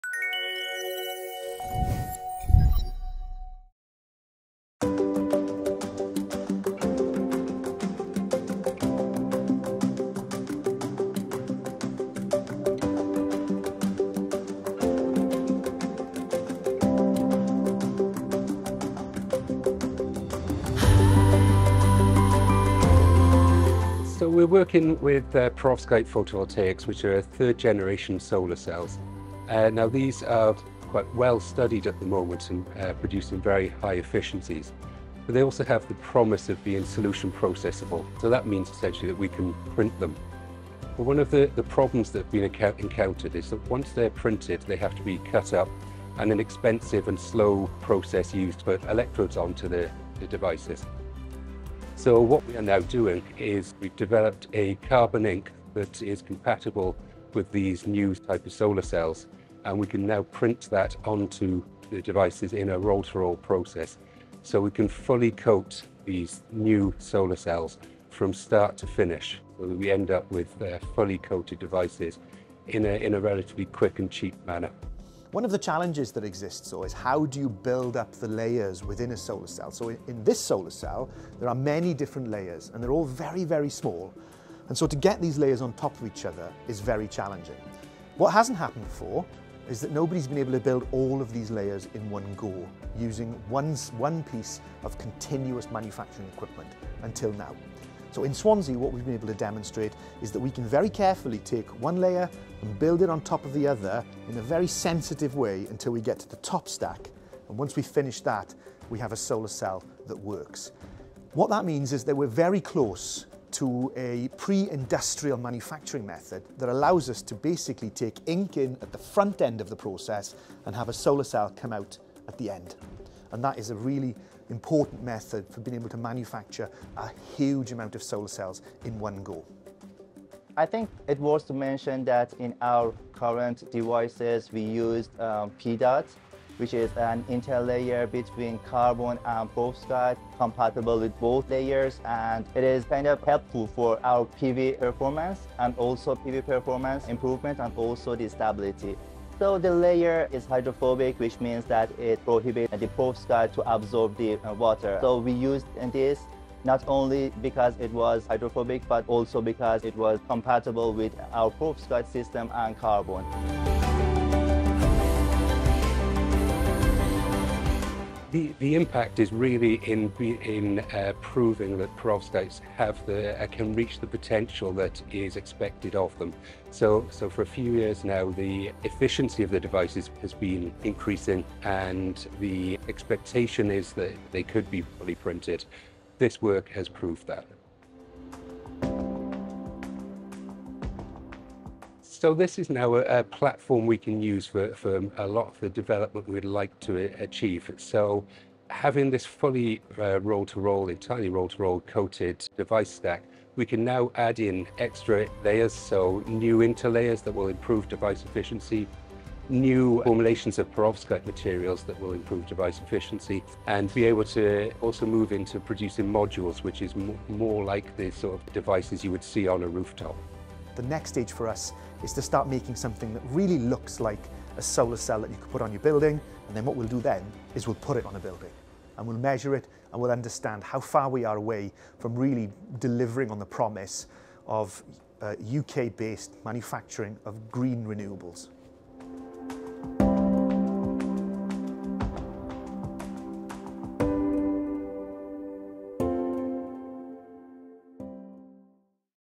World's First Genuine Fully Roll-to-Roll Printed Perovskite Solar Cell
Researchers at SPECIFIC Innovation & Knowledge Centre, Swansea University, have established a low-cost and scalable carbon ink formulation capable of unlocking the potential for perovskite solar cells to be manufactured at scale.

The team searched for an alternative to the gold electrode that is typically applied using an expensive and slow evaporation process after the device has been printed.

The new carbon electrode performs as well as gold and can be printed using slot die coating in a roll-to-roll (R2R) process.

They have created, for the first time, genuine “fully printable” perovskite photovoltaics (a term often used but, until now, incorrect).

Featuring Dr Dave Beynon, Dr Ershad Parvazian and Professor Trystan Watson.